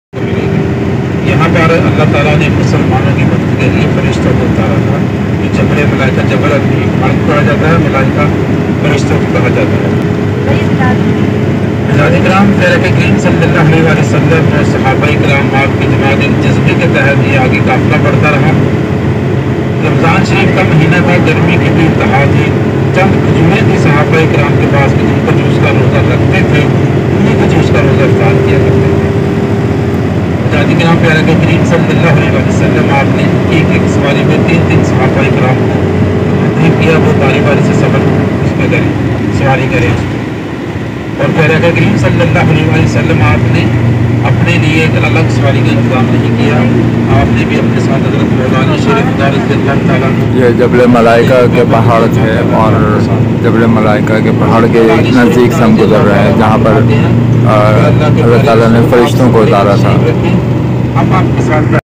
Mountains of Jabal Malaika, where Allah sent down the angels
جبل ملایئکہ کے پہاڑ جہاں اللہ تعالی نے فرشتے اتارے جہاں سے غزوہ بدر کا میدان بہت نذدیک ہے

Madina Makkah madinah makkahlive Mountains JabalMalaika Allah angels Ghazwahebadar
Allahuakbar MohammadPBUH Ground madinasharif makkahmadinah makkahwalk